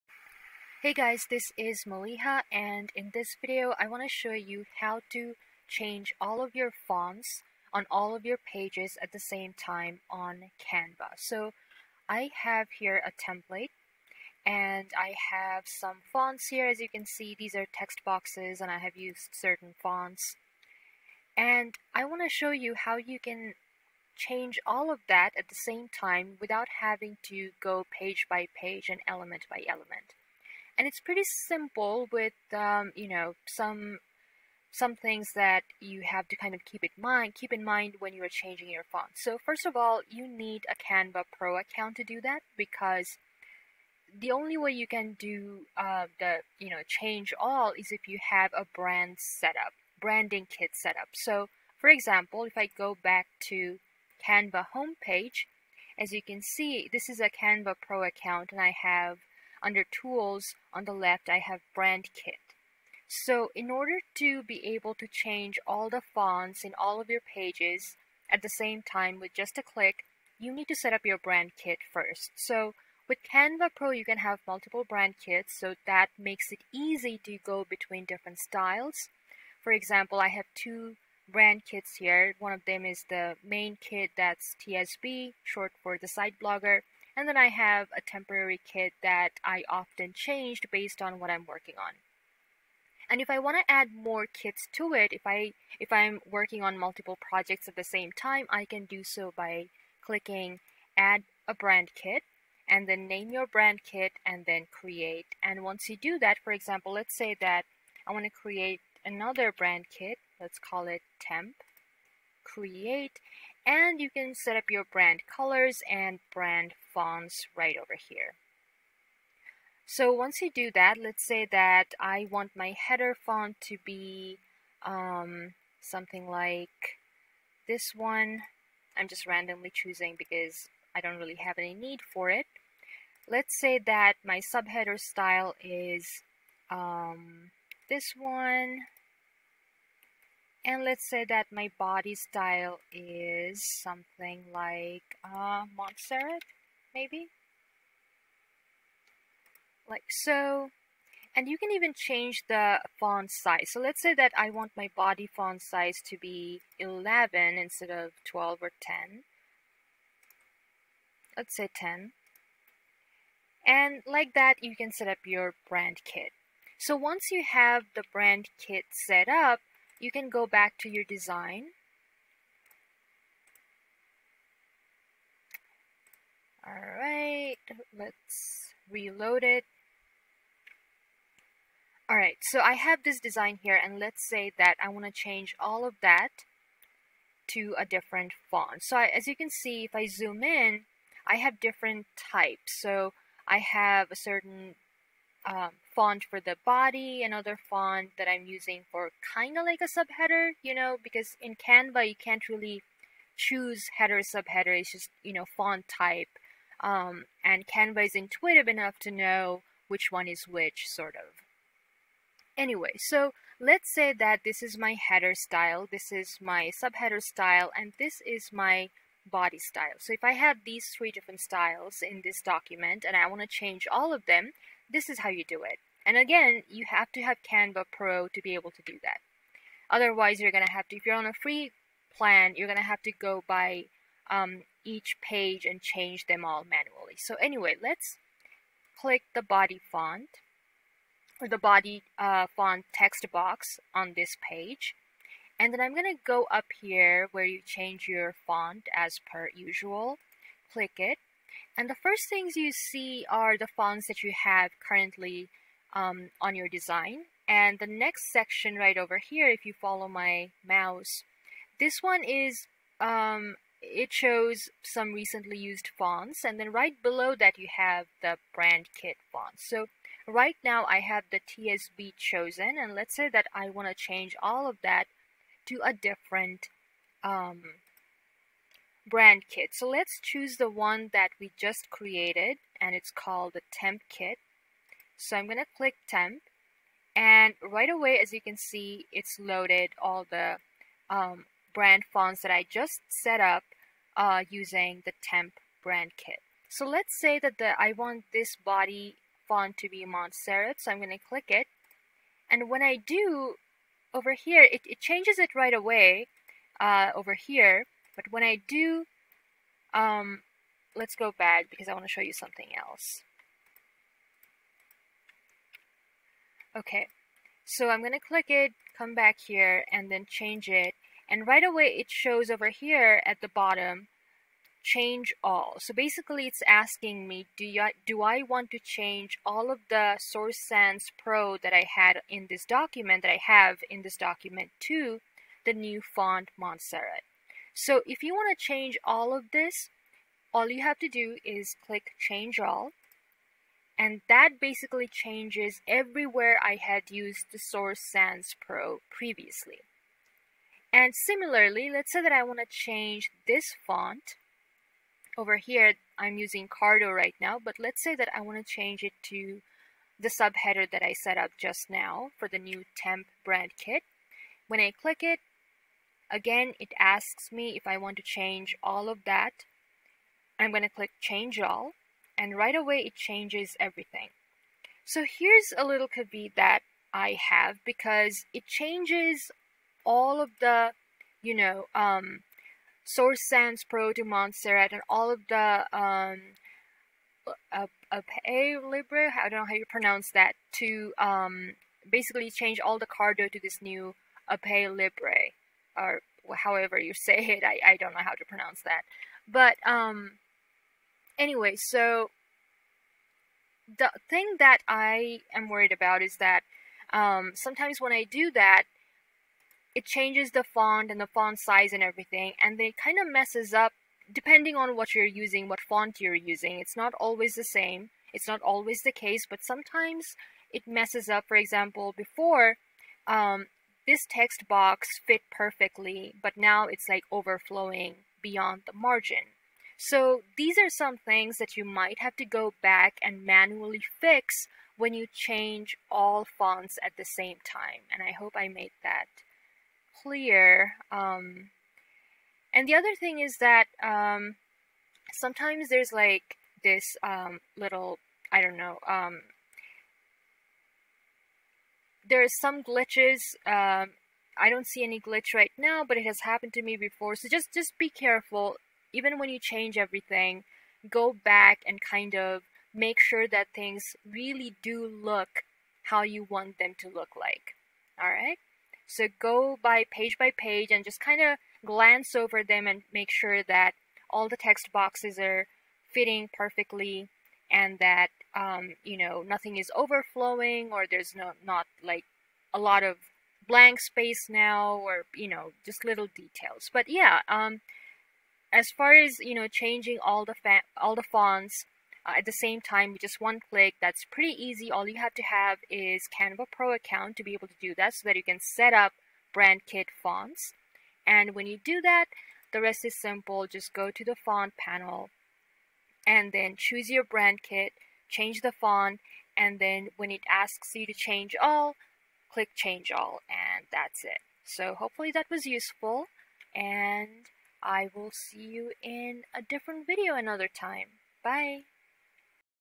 Canva (Pro): How to Change a Font in All Pages with One Click
Canva pro tutorial: A simple way to change all the similar fonts on all pages with just one click: A Canva pro feature.